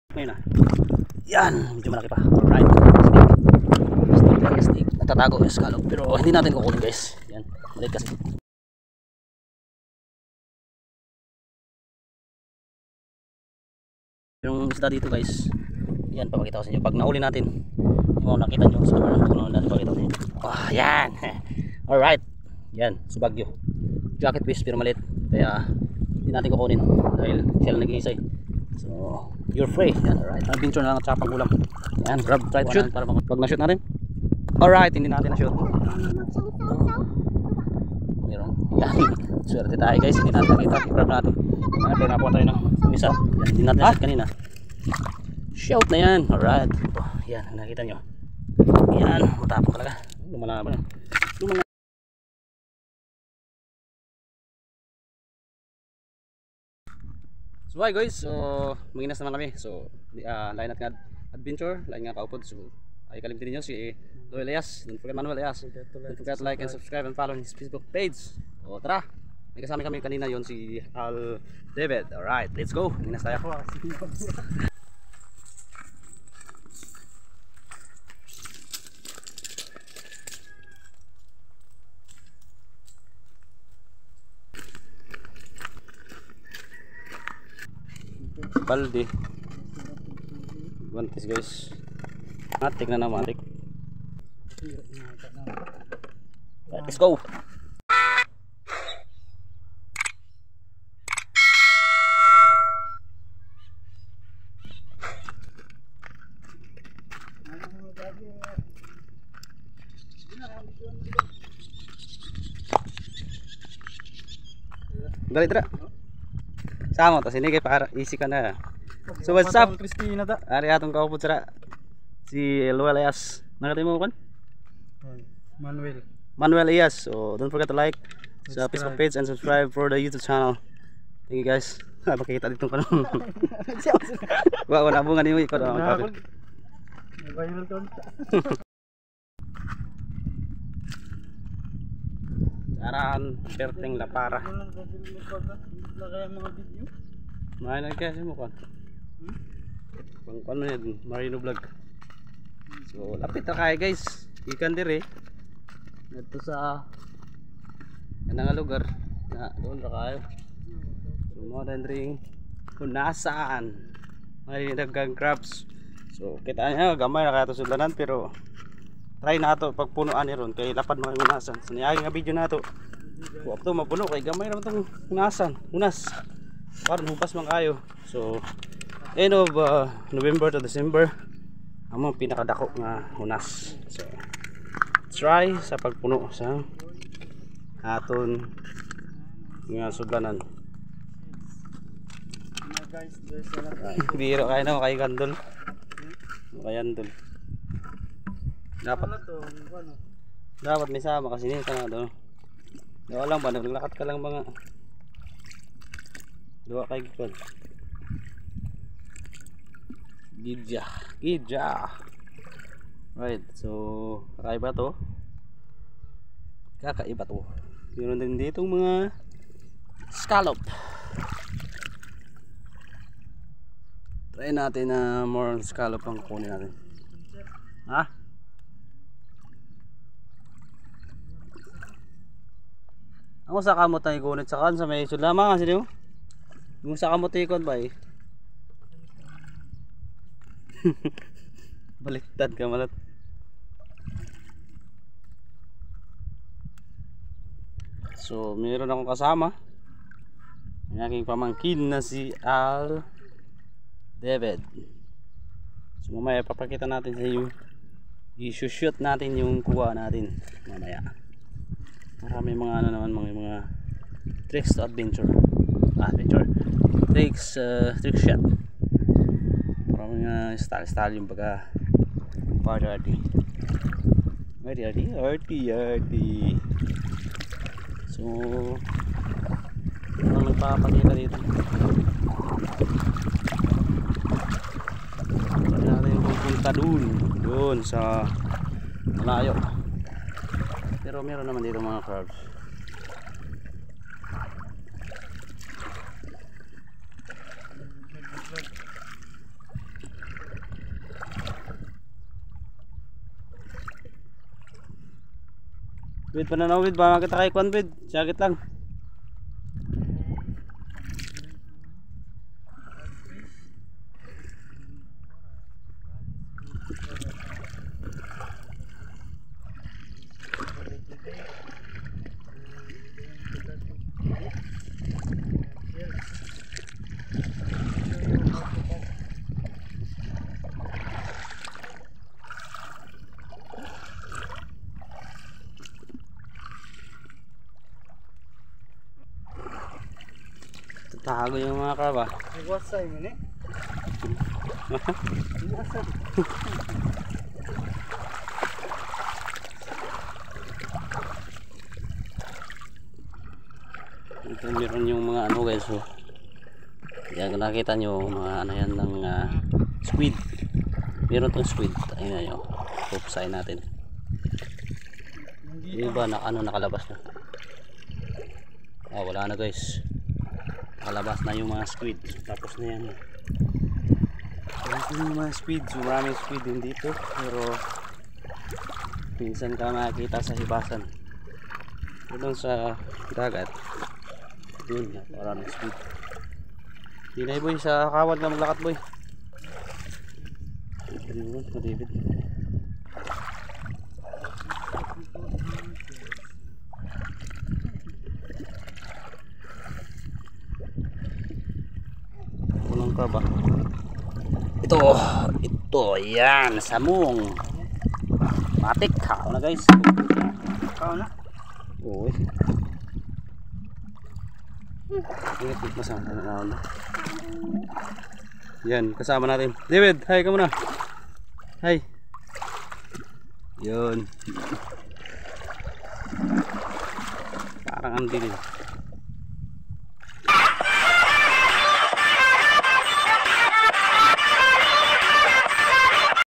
Tara panginas naman Adventure part 1 iba na Ang Kasama si manwel at aldivid.
Napaka raming scallop at crab dito sa aming Isla at Ang saya pa nood na para Makita mo Lalo na sa Taga city.